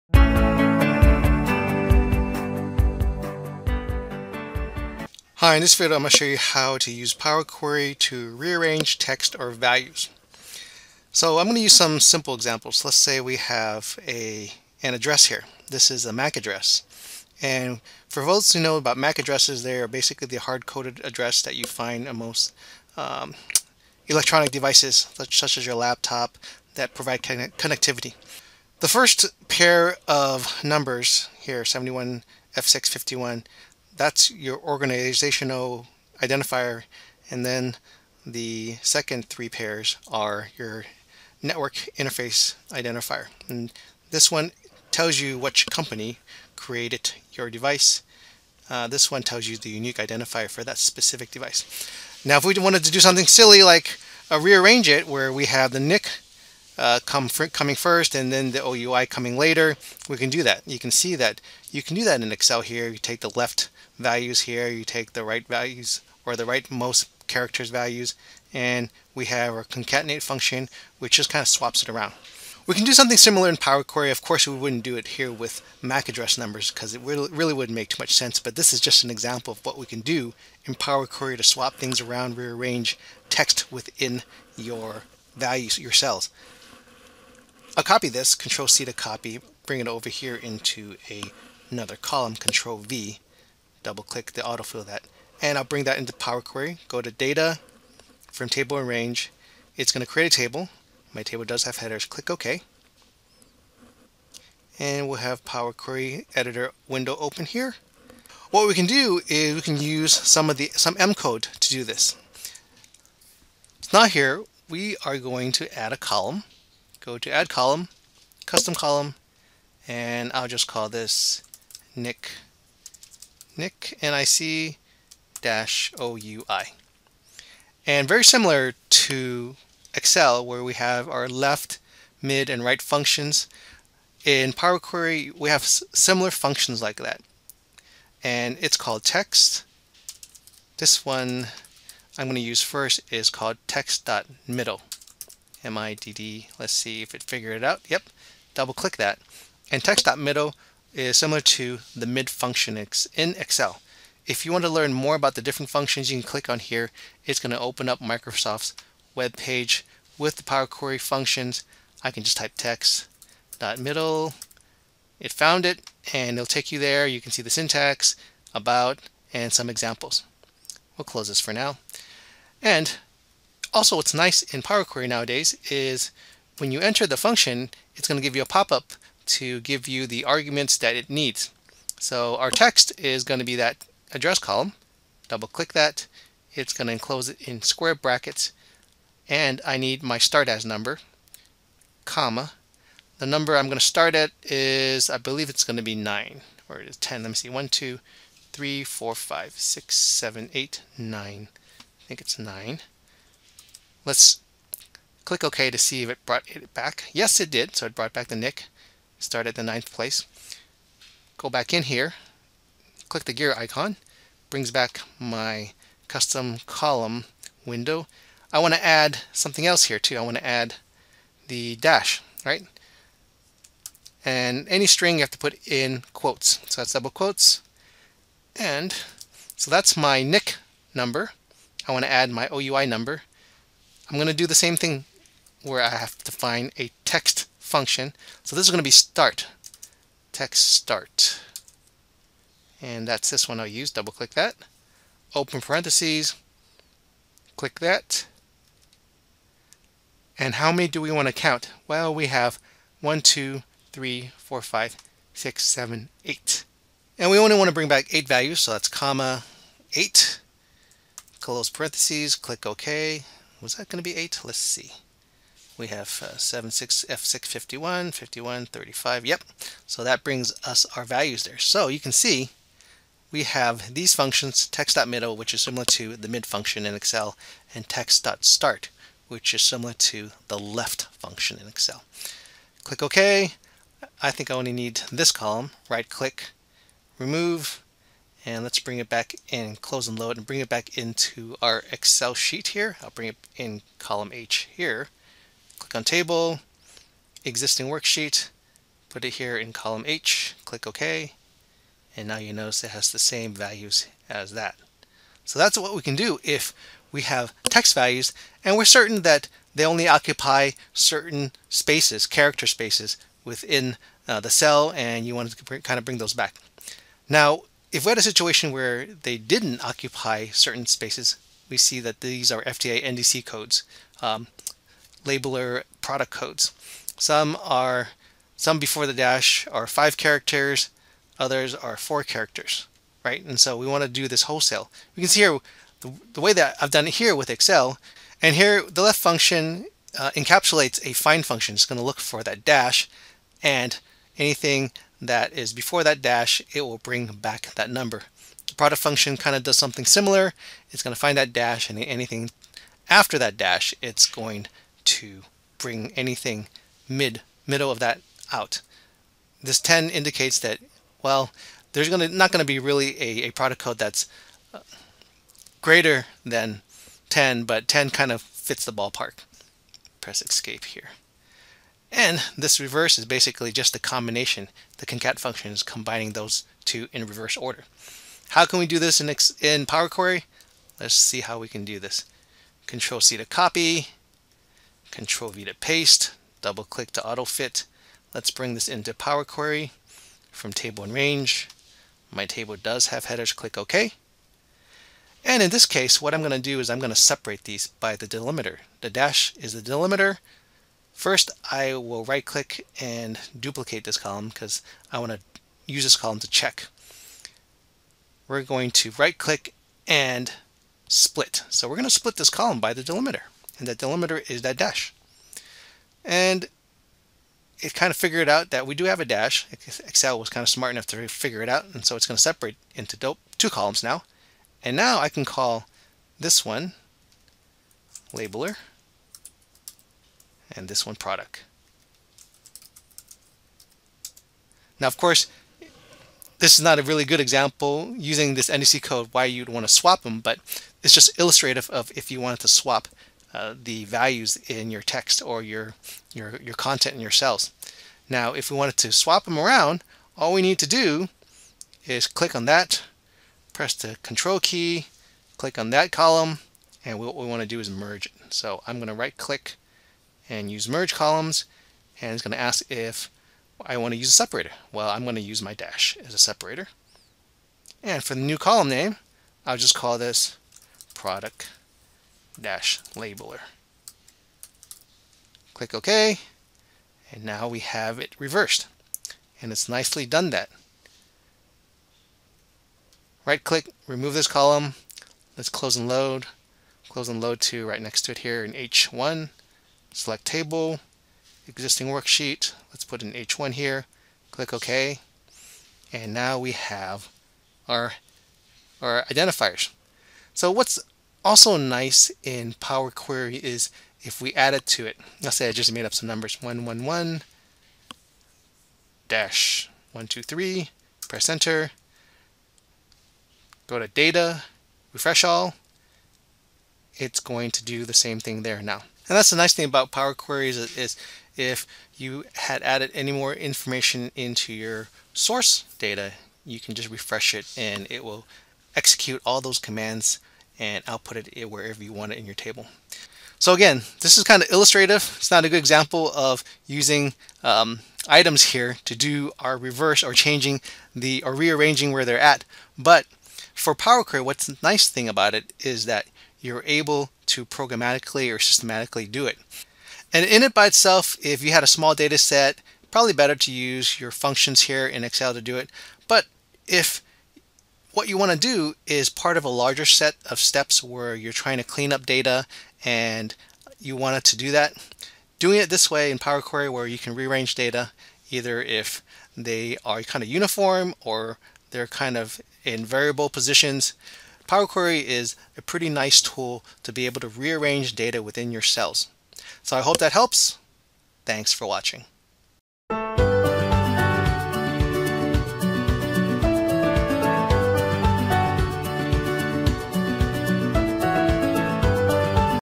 Use PowerQuery to Change the Order of Cell Values or Text
If you've tried to rearrange the order of text or data in cells within Excel, you're probably familiar with the LEFT, MID and RIGHT functions.  The combination of these functions help you reverse text or first and last names.  You can rearrange the data of almost any kind of content in your cells with these function.  Incidentally there are formulas in the Power Query M code that can do the same thing and this video will cover two examples.